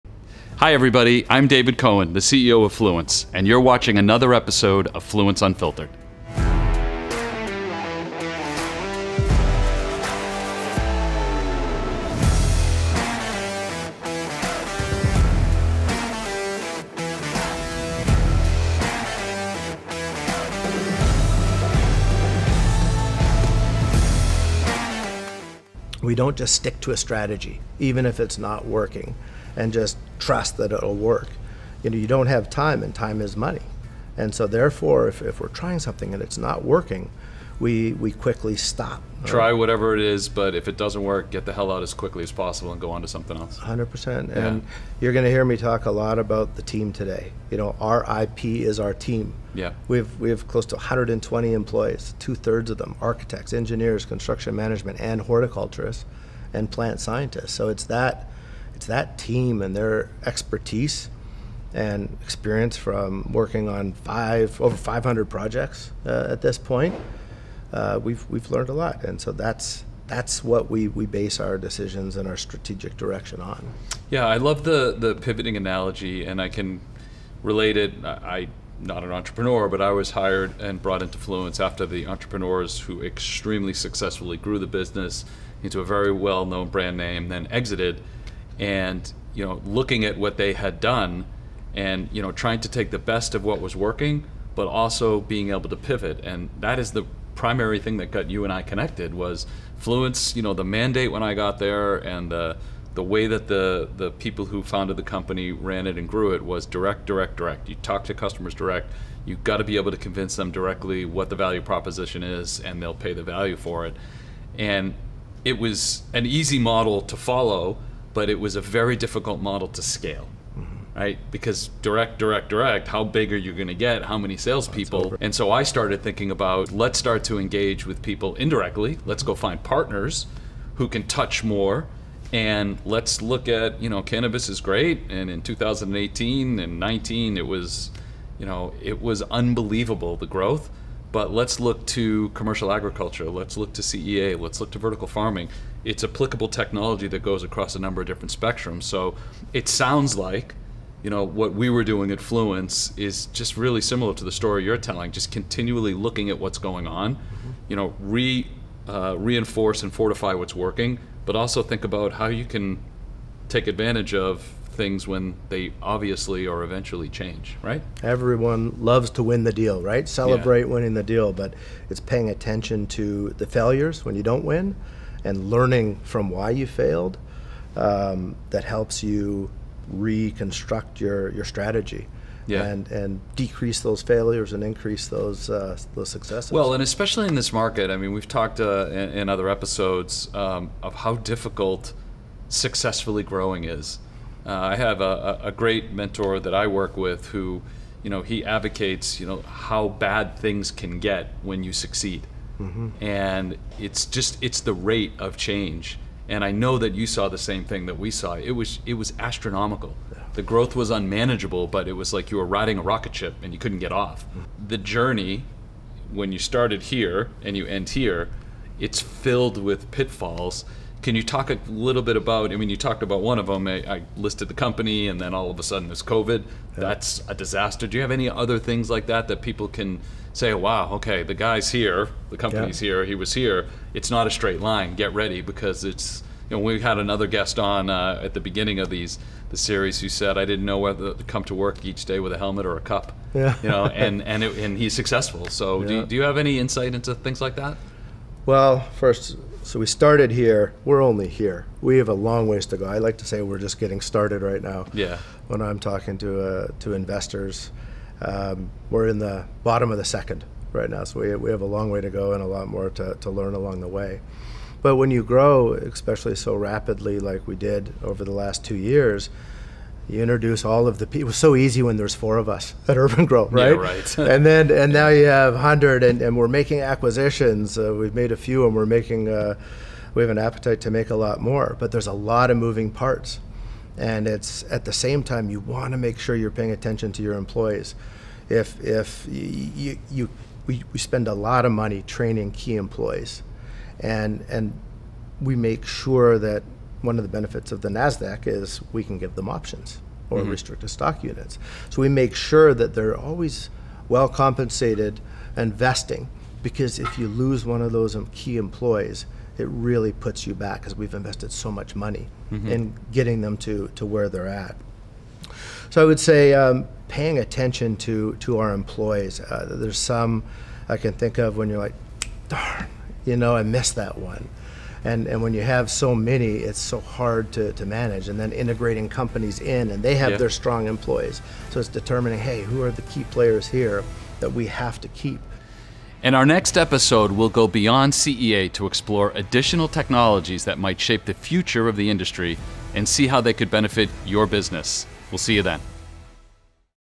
Fluence Unfiltered (ep.10): Scaling in the CEA Industry
In Episode 10, Fluence CEO David Cohen continues his conversation with Bradley Nattrass, the Chairman & CEO of urban-gro, Inc. Bradley goes into detail on how urgan-gro has become successful at what they do today in Controlled Environment Agriculture (CEA). Nattrass goes into great leadership learnings and how to successfully grow a business, including how they achieved the goal of getting urban-gro listed on the NASDAQ.